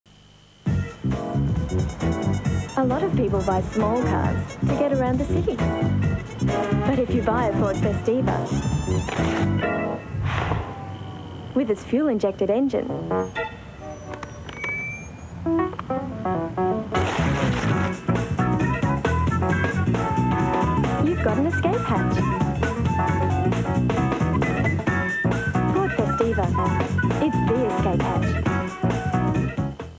Ford Festiva ad (1995)
From an airing of Hard to Kill on Nine Sydney.